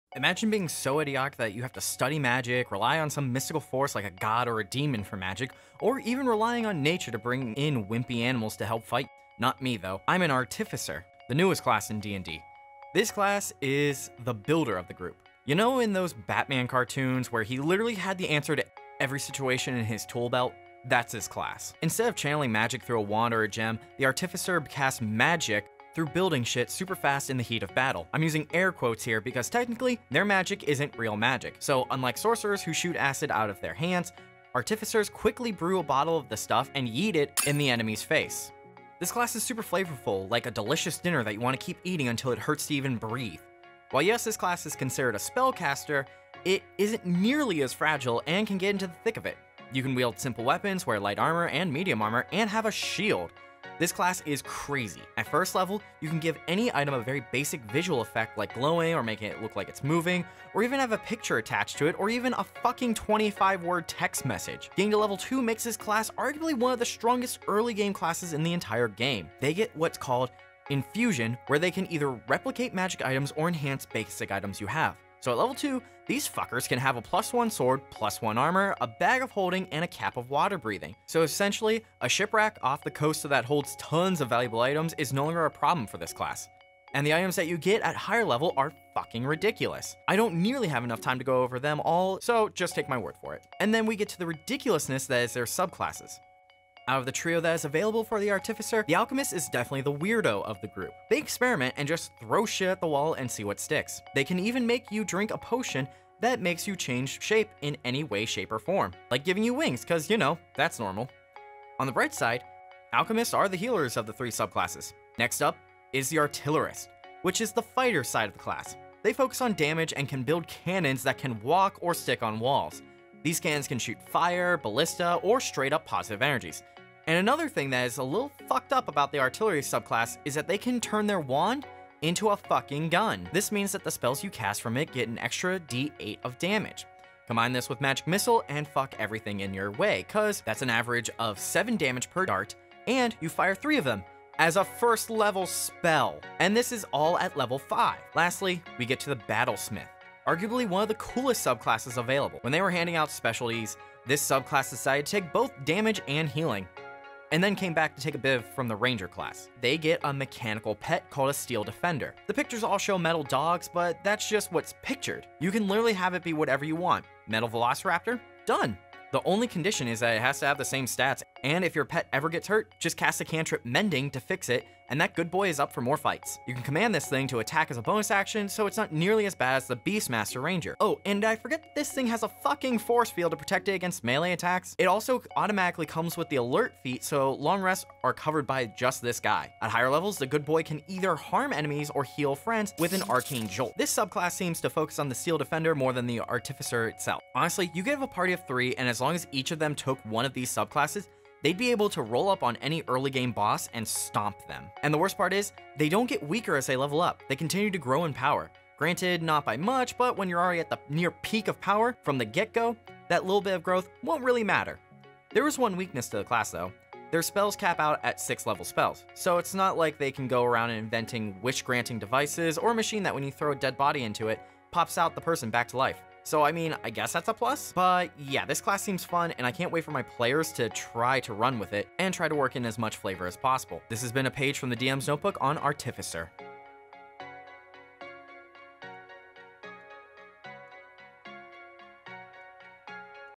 The DM's Notebook: Artificer
Wizards of the coast has decided that there needs to be a 13th class in Dungeons and Dragons and has come out with the Artificer, so ya know I gotta talk about.